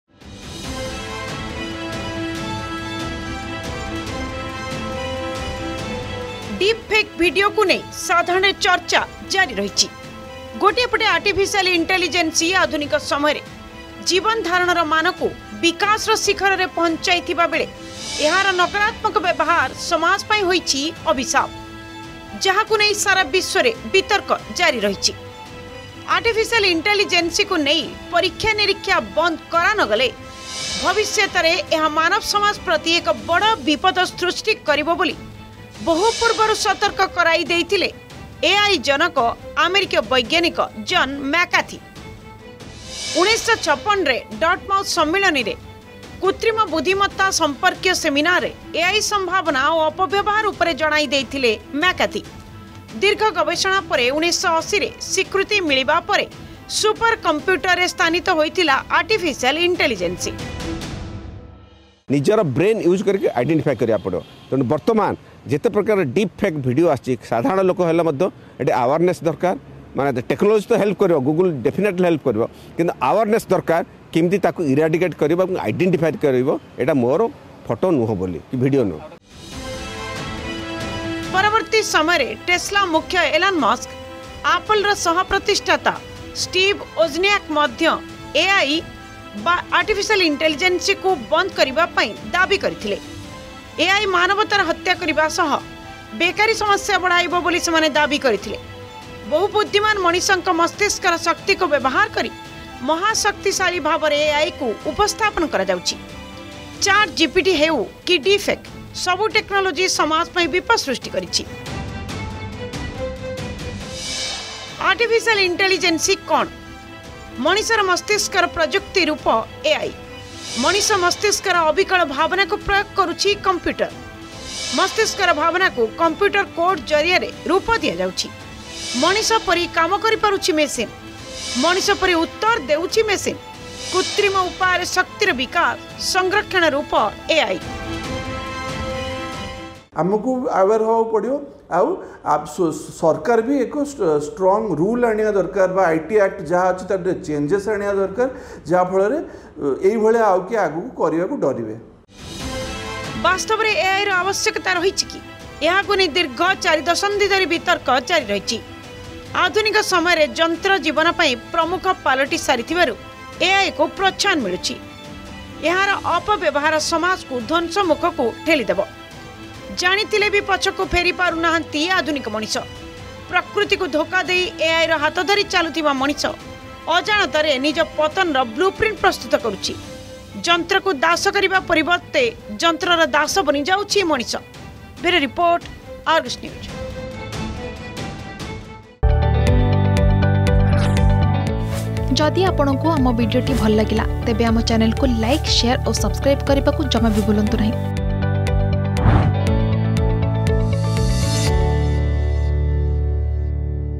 ନକରାତ୍ମକ ବ୍ୟବହାର ସମାଜ ପାଇଁ AI ହୋଇଛି ଅଭିଶାପ || Deepfake Videos
ଯନ୍ତ୍ରକୁ ଦାସ କରିବା ପରିବର୍ତ୍ତେ ଯନ୍ତ୍ରର ଦାସ ବନୁଛି ମଣିଷ । ଡିପଫେକ୍ ଭିଡିଓକୁ ନେଇ ସାଧାରଣରେ ଚର୍ଚ୍ଚା ଜାରି ରହିଛି । ଗୋଟିଏ ପଟେ ଆର୍ଟିଫିସିଆଲ ଇଣ୍ଟେଲିଜେନ୍ସ ଆଧୁନିକ ସମୟରେ ଜୀବନ ଧାରଣର ମାନକୁ ବିକାଶର ଶିଖରରେ ପହଞ୍ଚାଇଥିବାବେଳେ ଏହାର ନକରାତ୍ମକ ବ୍ୟବହାର ସମାଜ ପାଇଁ ହୋଇଛି ଅଭିଶାପ ।